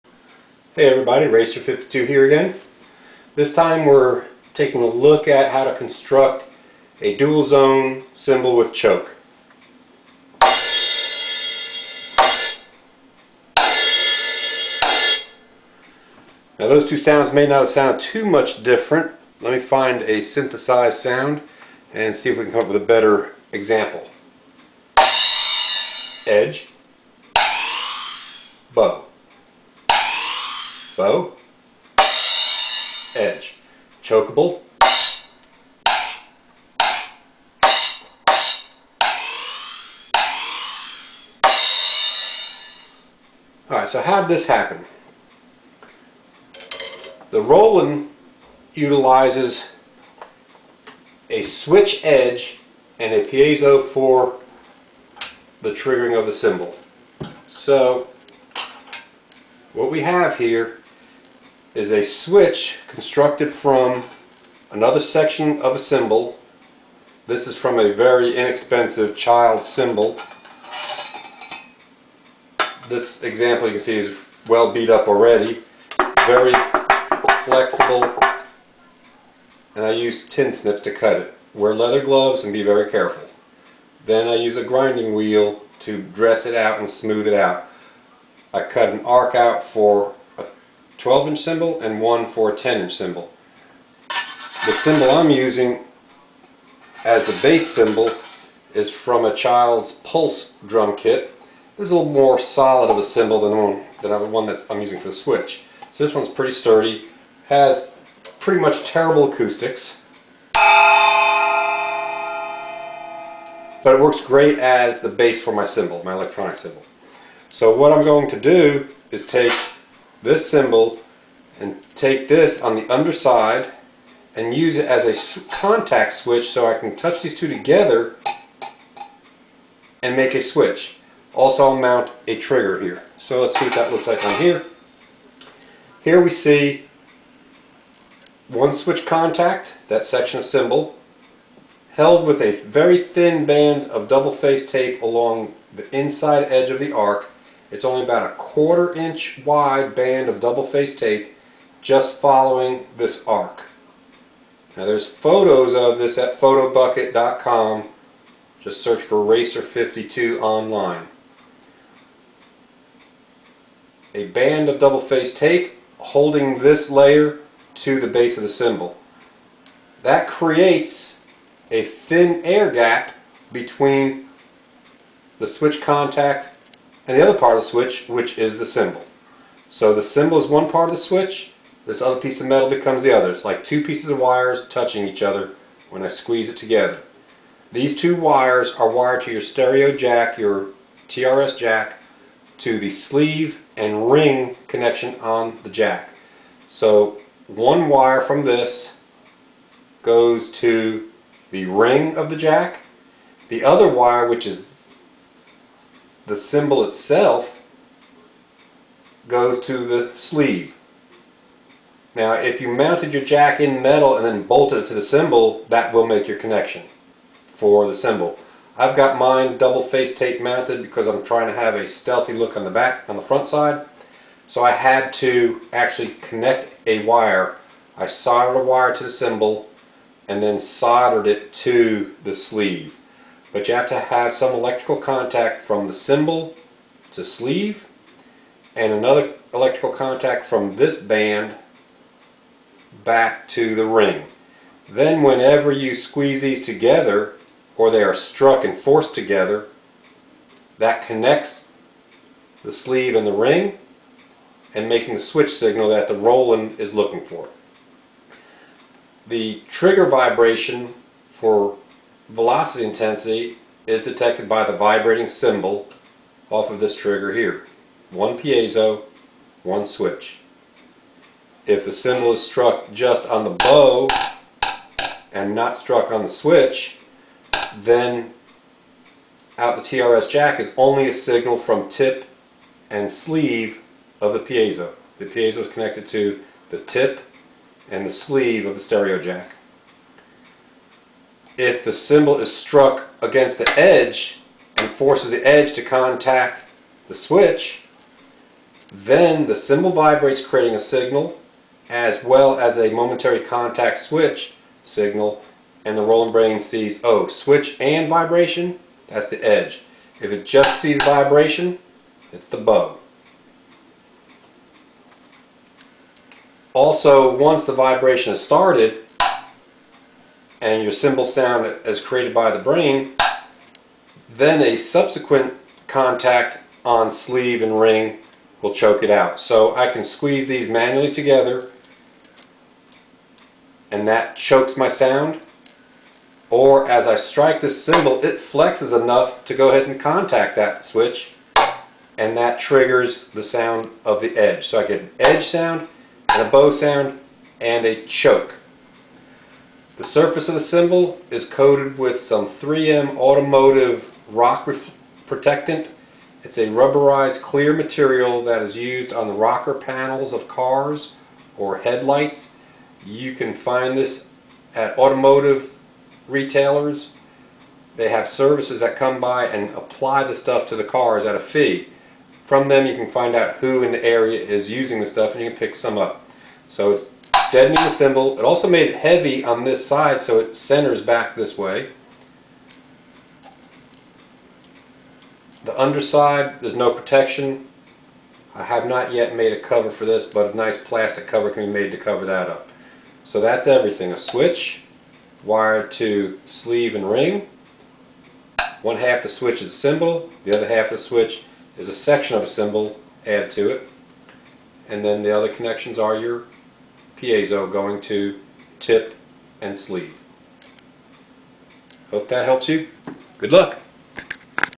DIY Dual Zone Cymbal with Choke.MPG
How to make a DIY Dual Zone Cymbal with Choke for a Roland drum module.
 This build uses a single Piezo and an edge switch. The switch is built from a section of another cymbal and mounted to the underside edge of the converted cymbal. The Cymbal makes one side of the switch and the arc strip of metal makes the other side of the switch. A piezo is wired to Tip & Sleeve of the TRS jack.  The two sides of the  "switch"  are wired to Sleeve and Ring of the TRS jack.